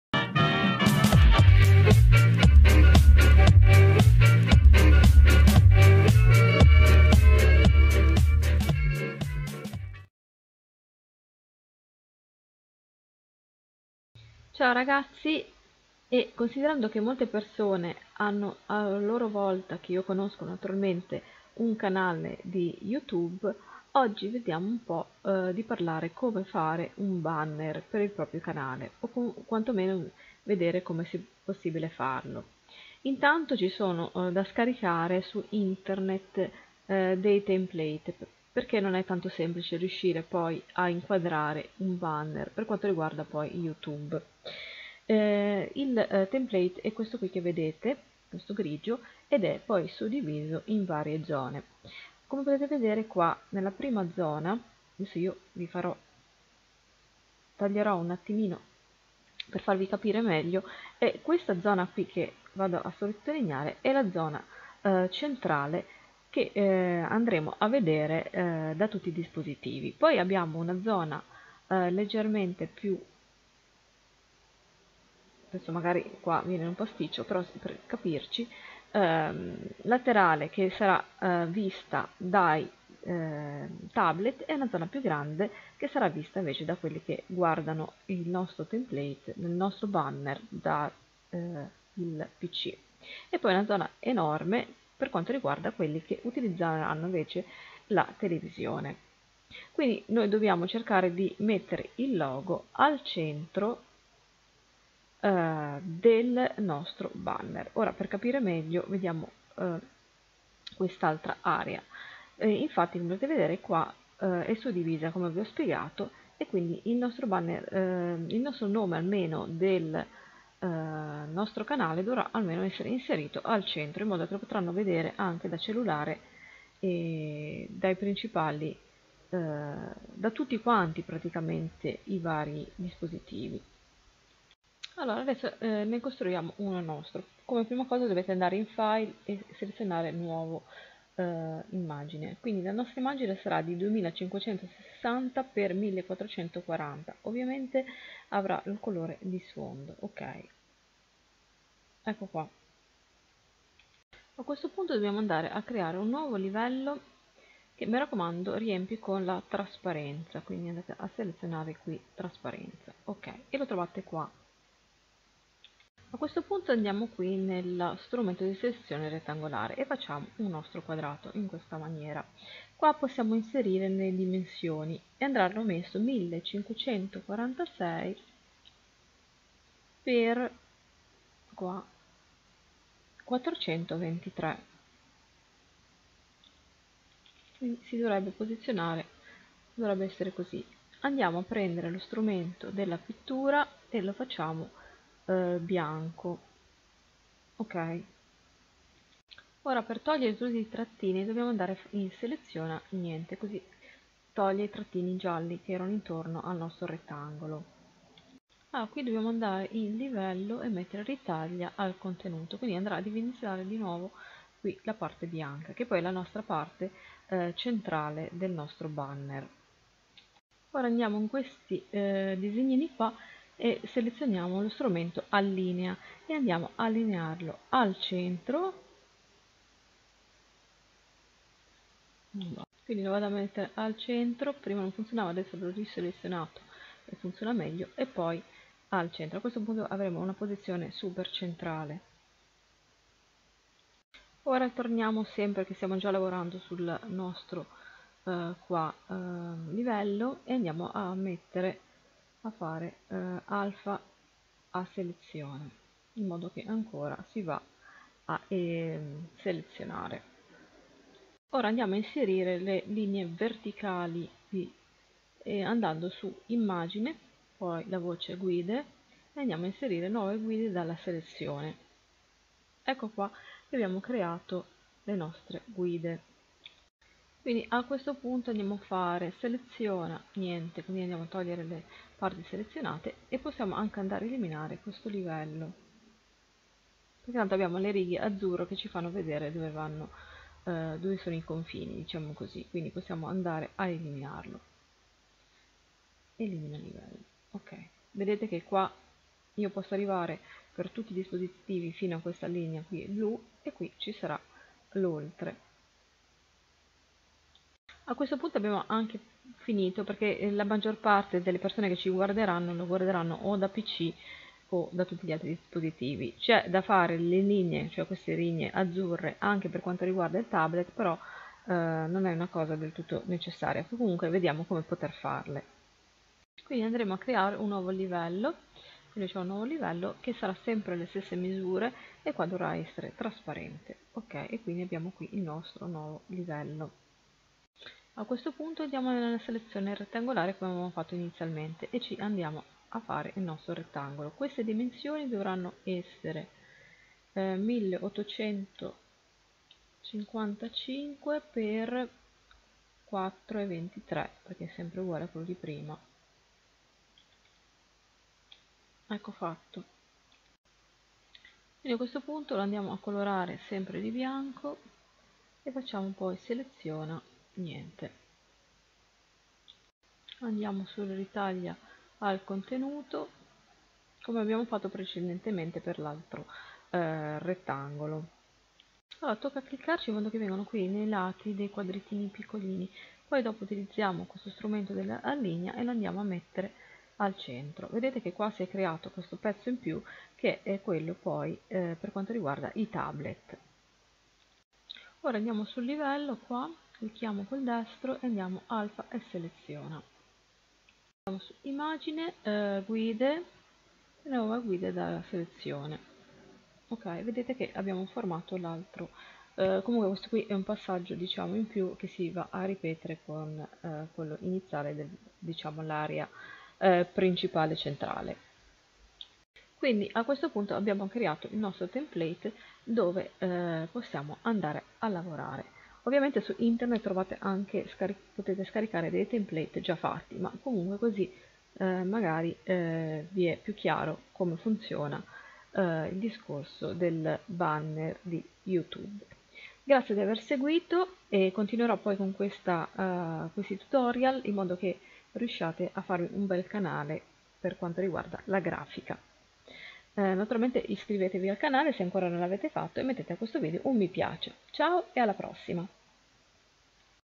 Come creare un template per banner di youtube GIMP ITA #1
Ciao ragazzi, oggi vediamo come creare il template per il banner di you tube. Userò il programma gratuito gimp, ma naturalmente potete usare anche photoshop.

Dimensione banner 2560 x 1440
1. Crea un nuovo livello trasparente
2. creare un rettangolo 1546x 423
3. usare il secchiello e colorare
4. seleziona niente
5. Livello auto ritaglio
6. usare il tasto allinea e posizionarlo al centro
7. sull immagine livello usare tasto destro e alfa selezione
8. immagine- guide- nuova guida della selezione.
9.seleziona niente.
10 nuovo livello
11. selezionare il rettangolo misure 1855 x 423
12. colorare con il colore
13. allineare al centro.